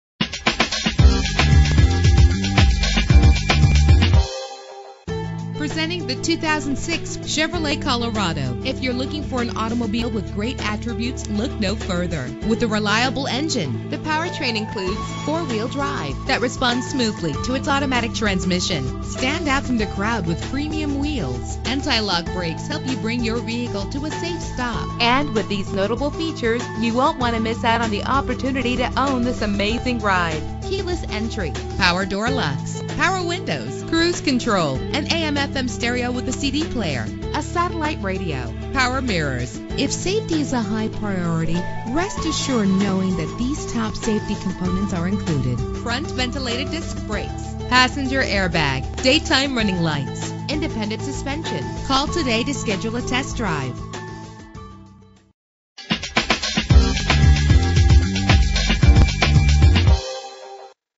2006 Chevrolet Colorado - Davenport FL
Year: 2006
Make: Chevrolet
Model: Colorado
Trim: LT
Engine: 3.5 liter inline 5 cylinder 20 valve
Transmission: 4-Speed Automatic
Color: Red
Mileage: 135526
Address:
42650 US Hwy 27